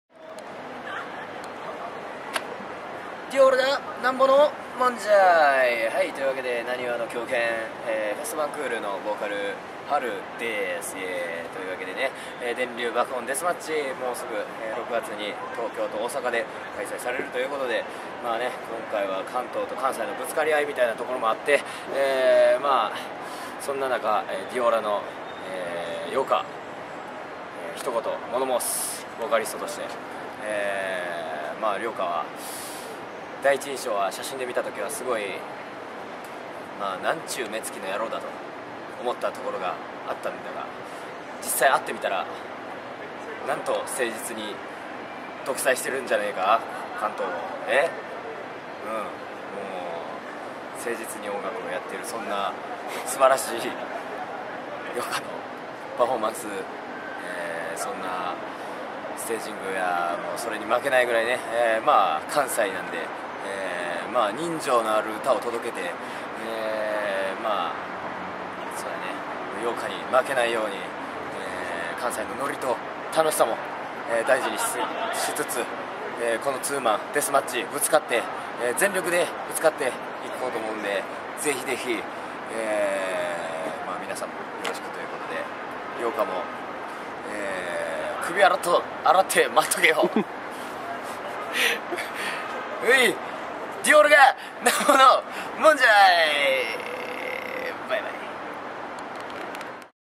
『FEST VAINQUEUR VS DIAURA～電流爆音デスマッチ～』チケット【一般発売】まであと9日!!
★2MAN LIVE【日程】2013年6月22日（土）【会場】umedaAKASO
【時間】開場16：30／開演17：00【料金】前売￥3,800（税込 D代別）
【出演】FEST VAINQUEUR/DIAURA
★チケット【一般発売】5月18日（土）10:00～
チケットぴあ：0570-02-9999  ローソンチケット：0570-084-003